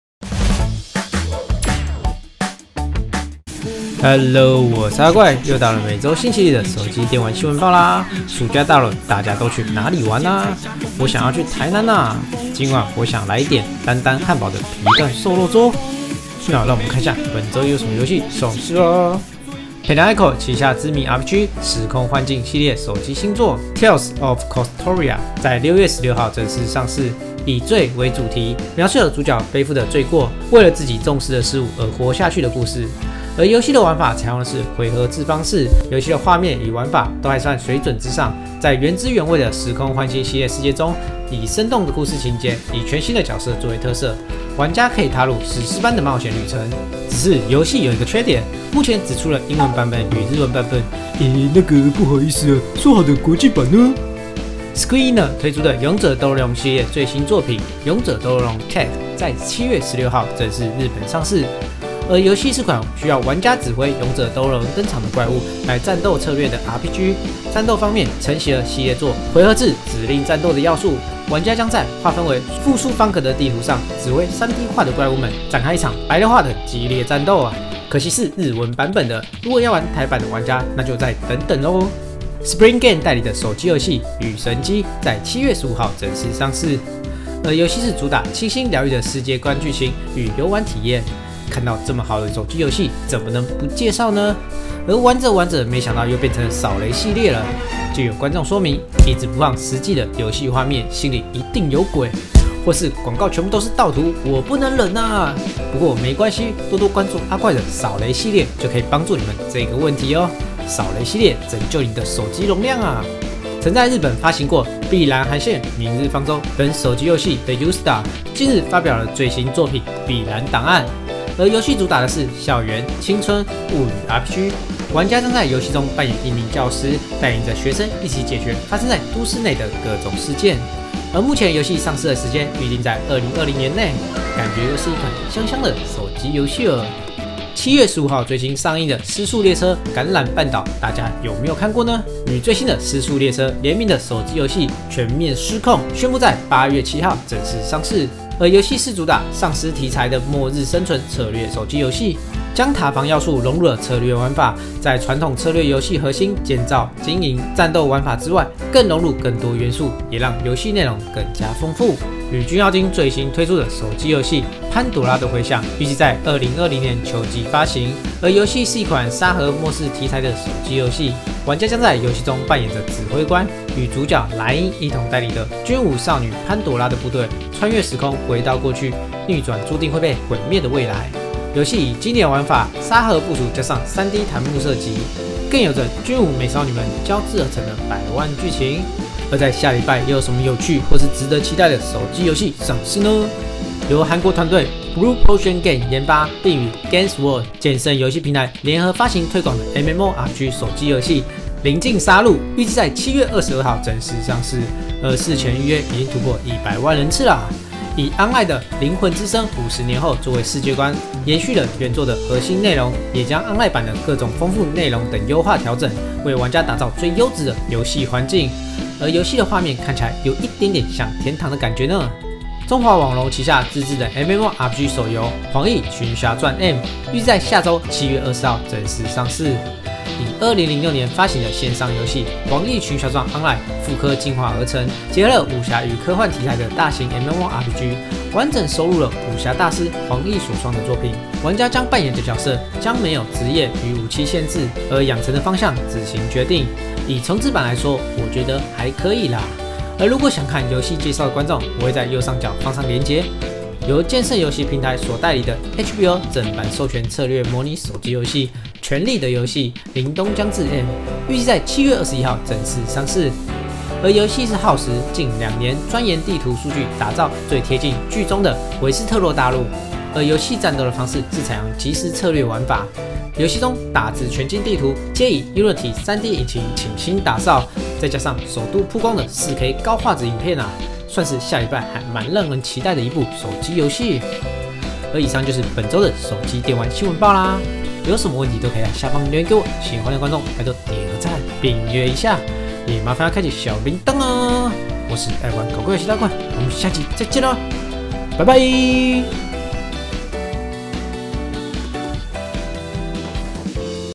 【20200719】手機電玩新聞報(新GAME資訊) feat 今晚想來點.....
三分鐘知道一周手機電玩資訊

本周上市遊戲介紹
《Tales of Crestoria》
《御神姬》
最近手機新聞
《碧藍檔案》
《全面屍控》
《潘朵拉的迴響：Echoes of Pandora》

下周可以期待手機遊戲
《靈境殺戮》